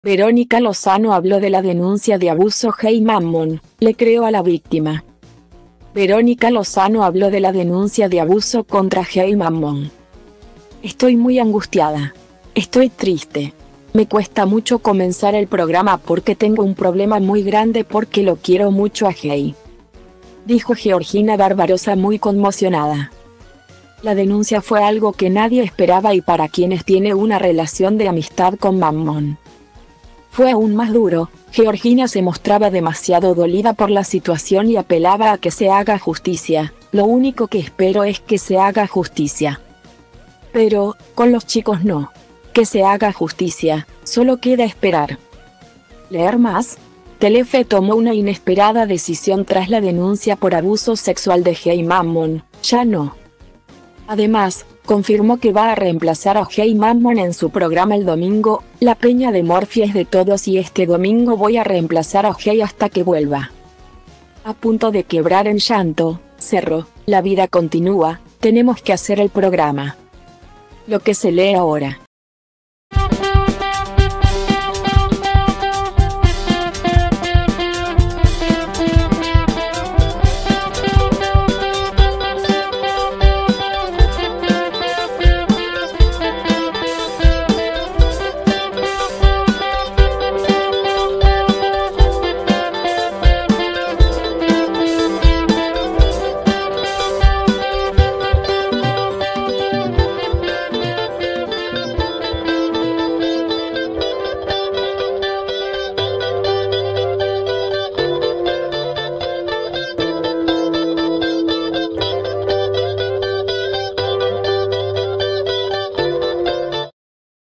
Verónica Lozano habló de la denuncia de abuso Jey Mammon: Le creo a la víctimaAr5
Verónica Lozano habló de la denuncia de abuso  Jey Mammon: Le creo a la víctima
Verónica Lozano habló de la denuncia de abuso contra Jey Mammon. “Estoy muy angustiada. Estoy triste. Me cuesta mucho comenzar el programa porque tengo un problema muy grande porque lo quiero mucho a Jey”,dijo Georgina Barbarossa muy conmocionada.